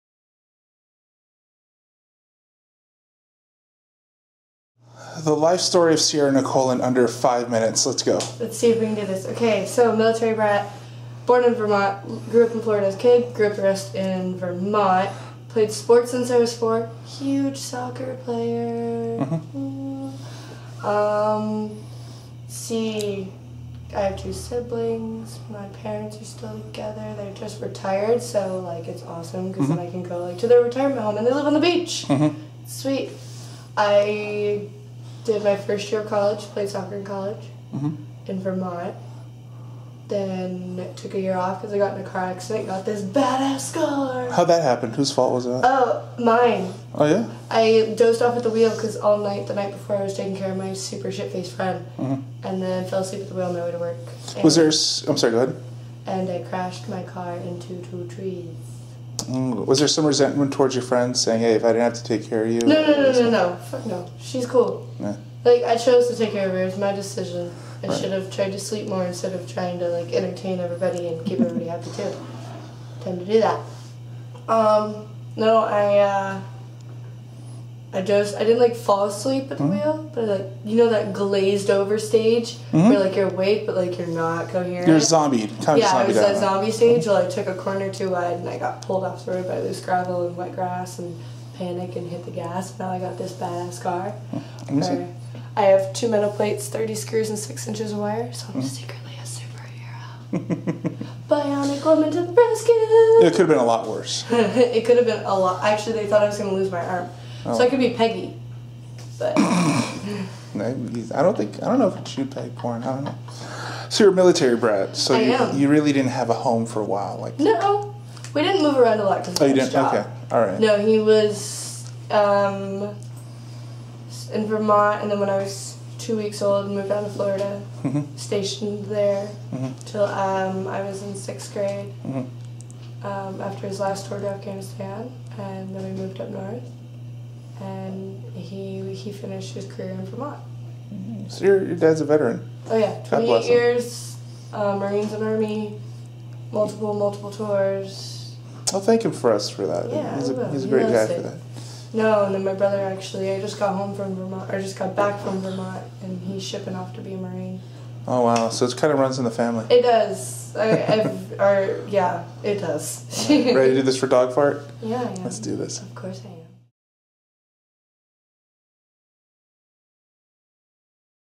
Introducing porn star Sierra Nicole
We loved having Sierra Nicole on set for Dogfart Video Productions. What did she have to say before her scene with Mandingo? Enjoy!